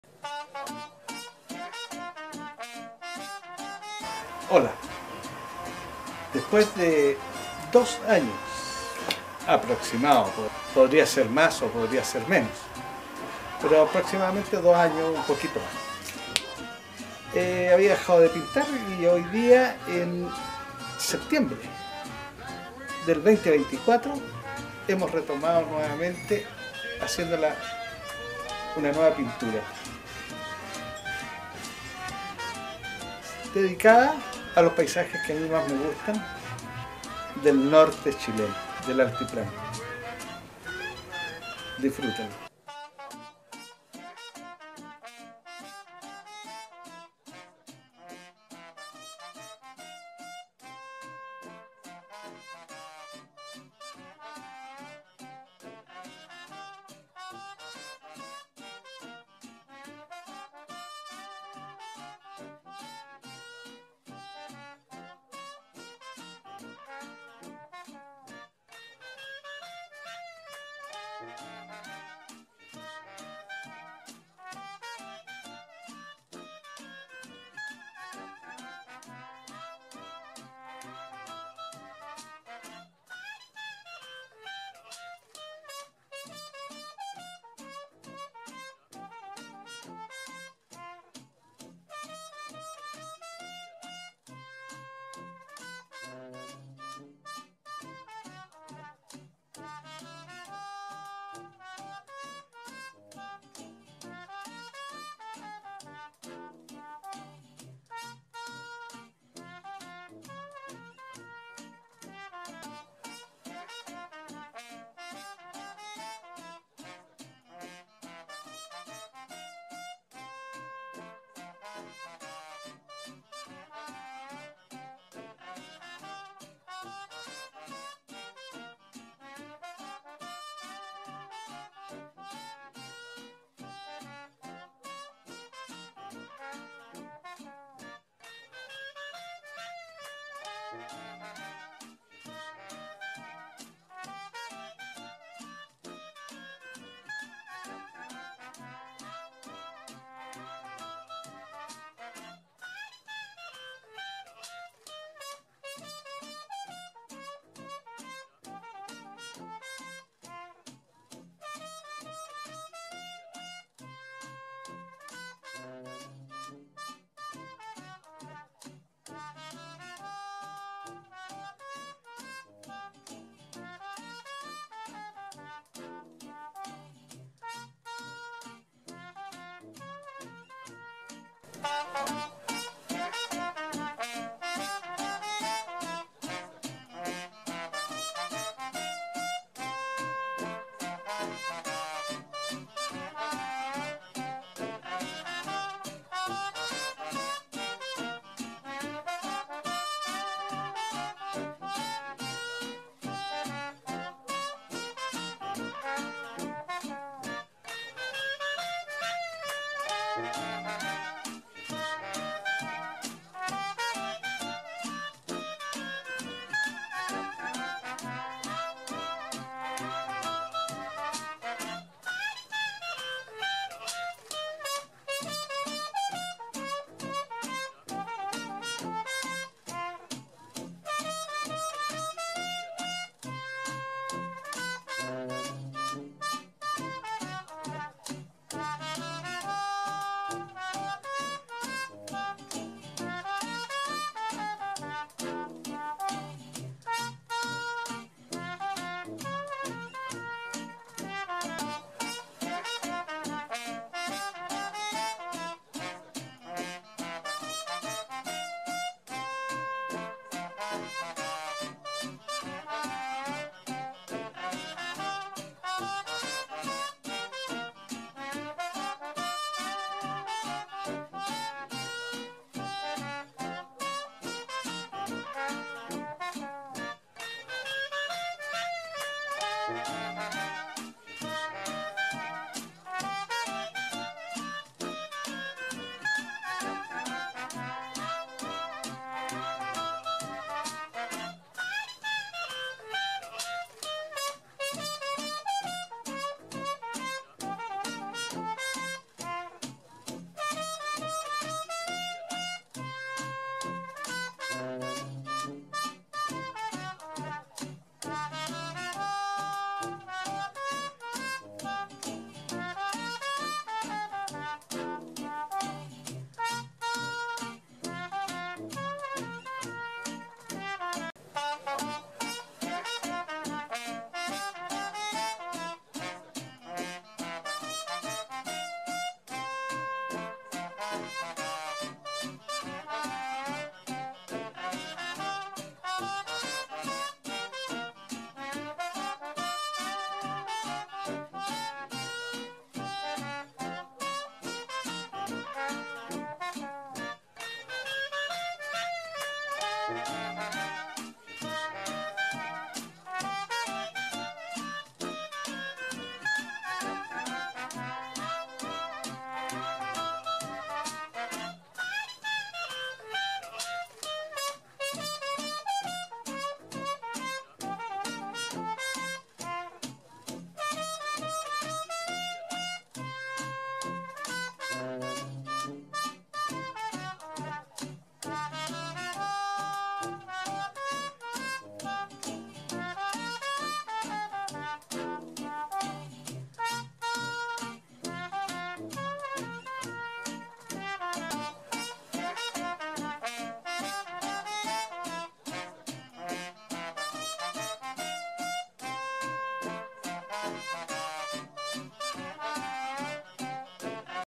Video 17de50 OLEO en SEPT 2024 de 24cm X 35cm - óleo en madera
MOISETON, Este seria ya mi  video numero 17 asociado igualmente a 17 intentos de pintar con óleo, en SEPTIEMBRE 2024